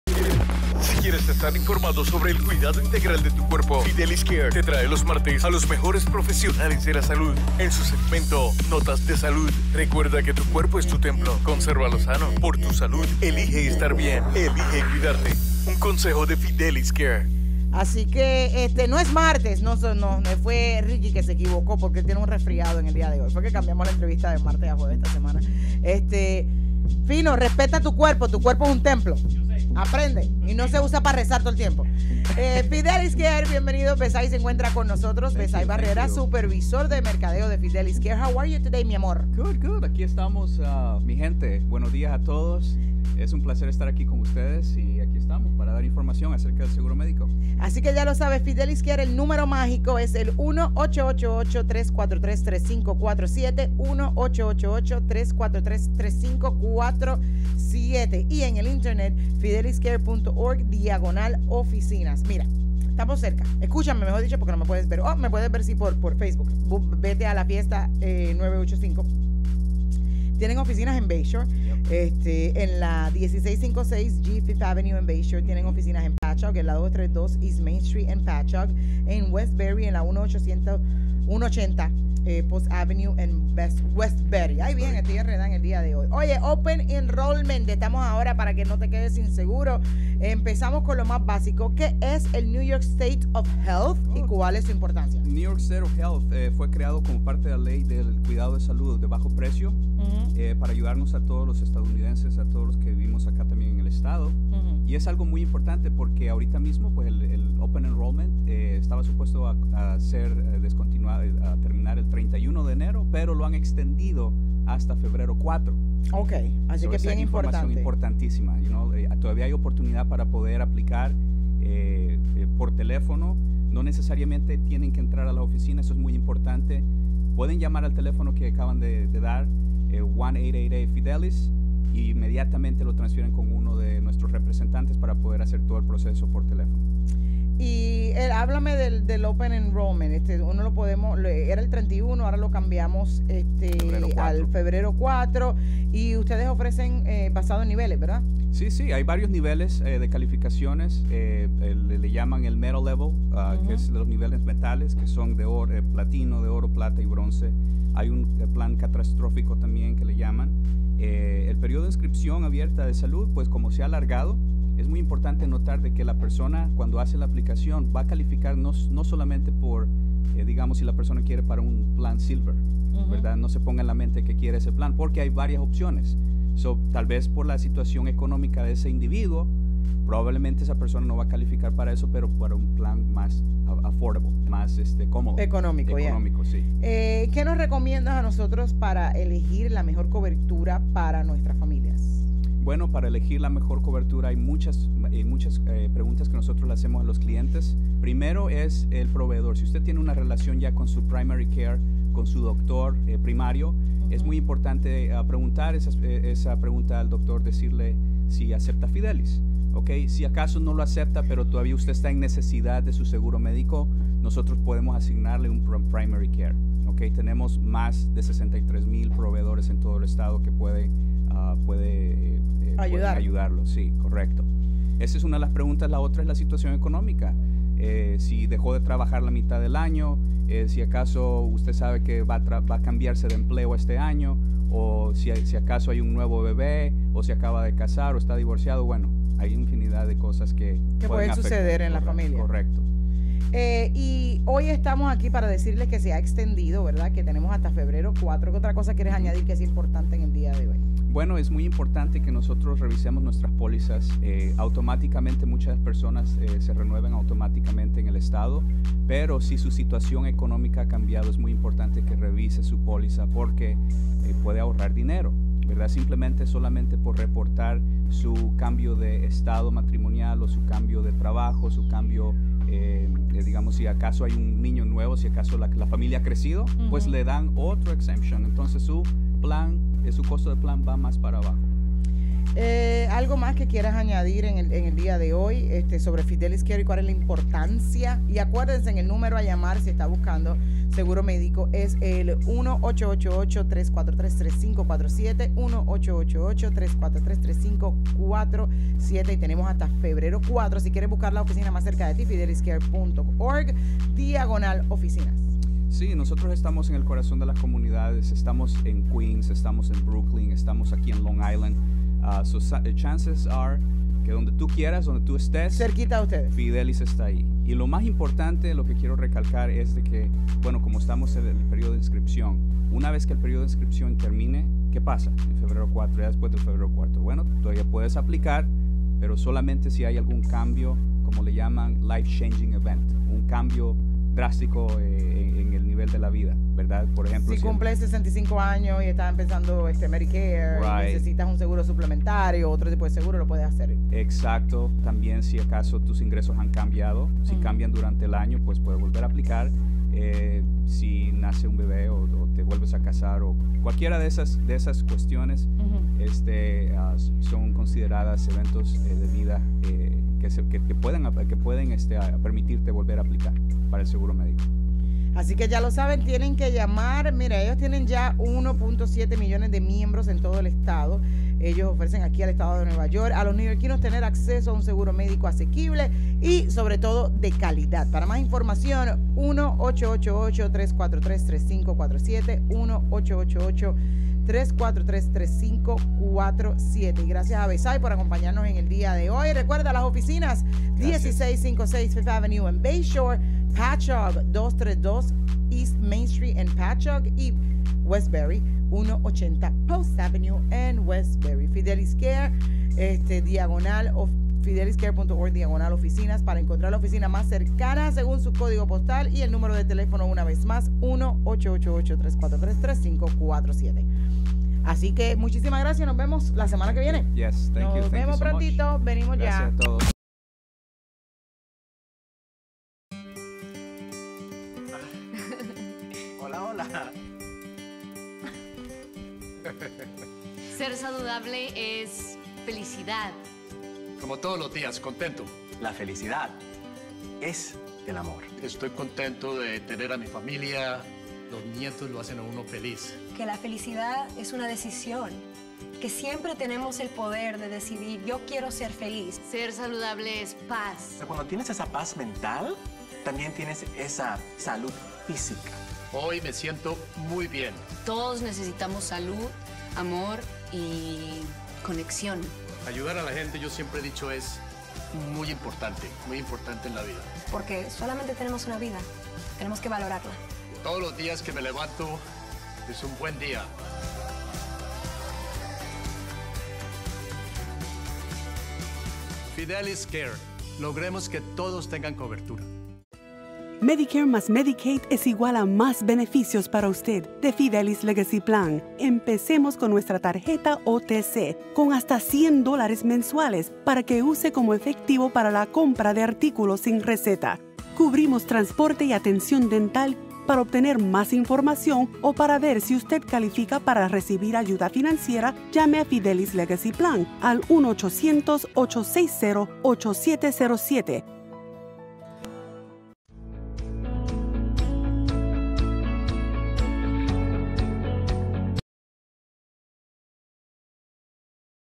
Planes de Salud en New York Por Fidelis Care
Hasta el 4 de febrero, 2020 se extiende el período de inscripción (Open Enrollment) para planes de Salud en el estado de New York.